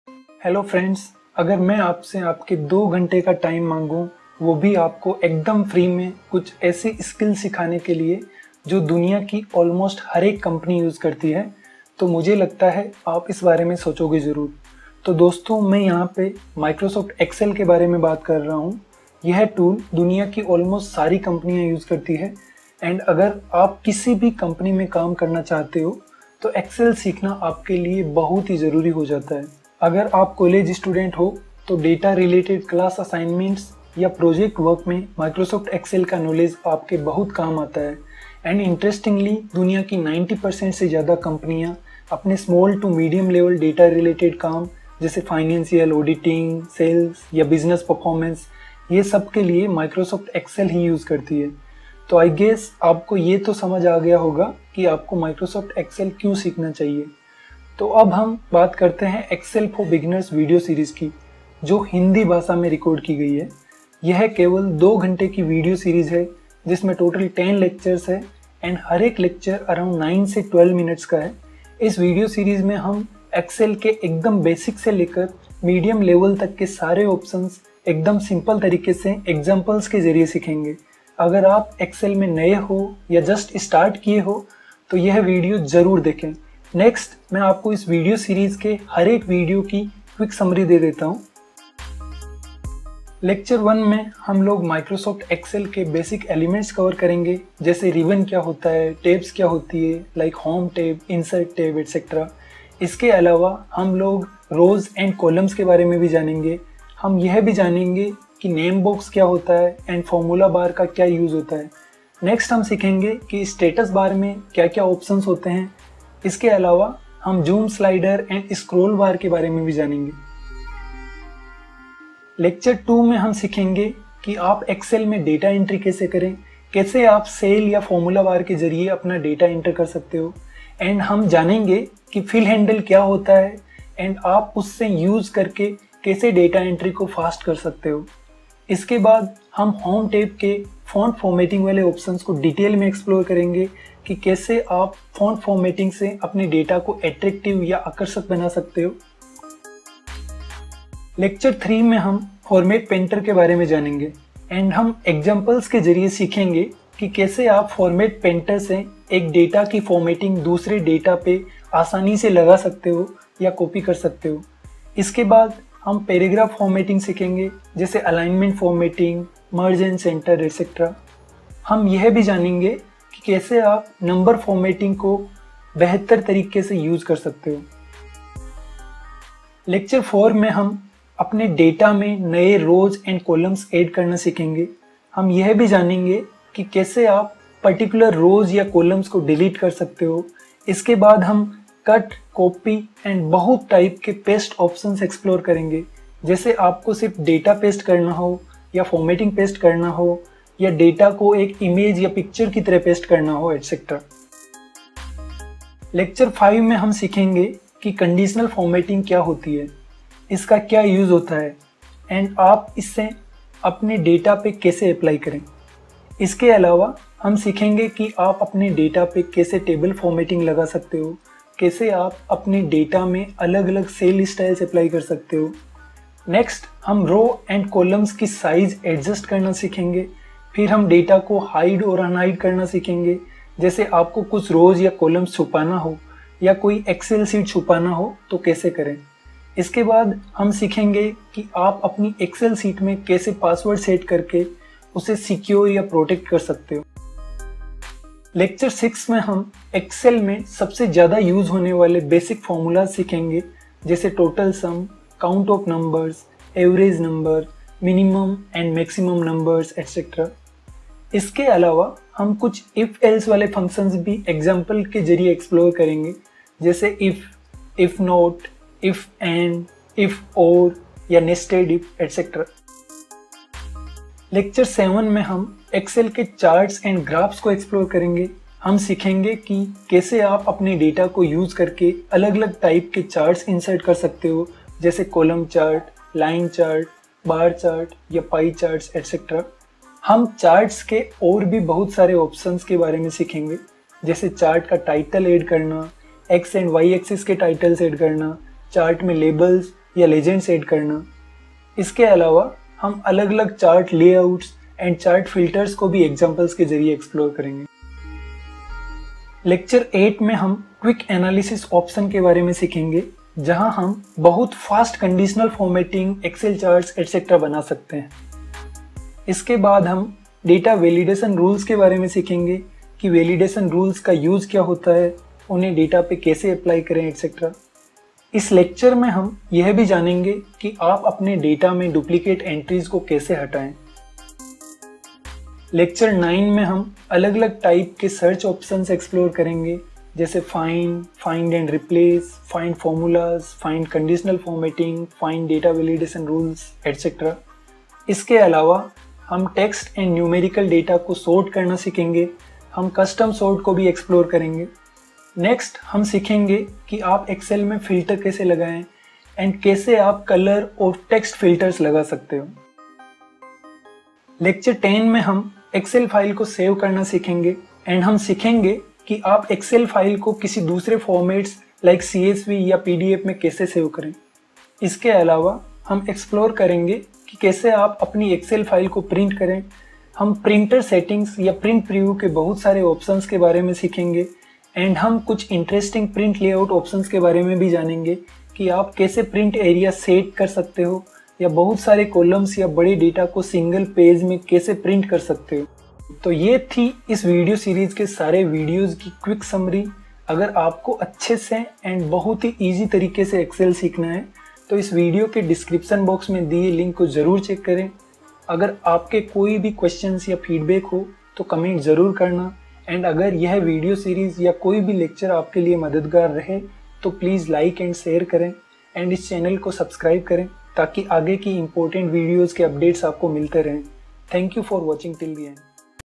Lecture 0: Microsoft Excel - Introduction | Complete Excel Tutorial for Beginners in Hindi
यह Excel For Beginners Video Series का Introduction Lecture है। इस वीडियो में हम Microsoft Excel और इस Video Series के बारे में जानेंगे।

Best Experience के लिए आप इस Video को 1080p [HD] Quality में देखे।

आप इस Video में बताए गए Major Topics नीचे देख सकते हैं।  यदि आप किसी Topic पर सीधे जाना चाहते हो तो उस Topic के Left में दिए हुए Timestamp पर क्लिक करें।

List of All Major Topics with TimeStamp [⌚]:
0:00  Introduction
0:15  Why should you Learn Microsoft Excel?
0:57  Excel for Beginners - Summary
1:31  Lecture 1: Microsoft Excel - Basics
2:00  Lecture 2 : Microsoft Excel - Data Entry and Formatting
2:29  Lecture 3 : Microsoft Excel - Format Painter, Alignment & Number Settings
2:57  Lecture 4: Microsoft Excel - Insert, Delete and Cut, Copy & Paste Settings
3:24  Lecture 5: Microsoft Excel - Conditional Formatting and Cell Formatting
4:07  Lecture 6: Microsoft Excel - Basic Functions and Formulas
4:32  Lecture 7: Microsoft Excel - Bar, Pie, Line Charts and Graphs
5:07  Lecture 8: Microsoft Excel - Quick Analysis, Data Validation, Comments and Notes
5:33  Lecture 9: Microsoft Excel - Data Search, Sort and Filter Options
6:02  Lecture 10: Microsoft Excel - Save File and Print Options

- अगर यह वीडियो आपके लिए Helpful रहा तो Please इसे Like और Share करें
- आपके सुझाव या Feedback मेरे लिए बहुत Important है!! यदि आपके कोई भी Questions या सुझाव हैं तो Please Comment जरूर करें